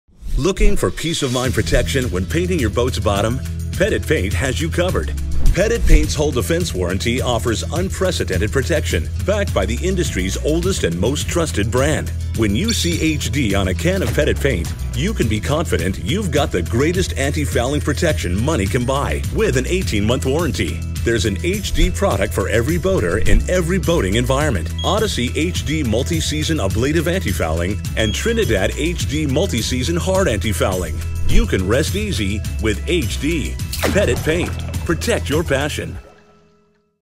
Pettit Paint - HD Hull Defense 18 Month Warranty Information
When your bottom paint fouls prematurely and hinders performance, you could spend hundreds or thousands of dollars in a short haul and fresh bottom paint job. Not with Pettit Paint! For the first time ever, if your bottom paint loses its effectiveness prior to 18 months, you can be covered by Pettit Paint's HD Limited Warranty. for more details.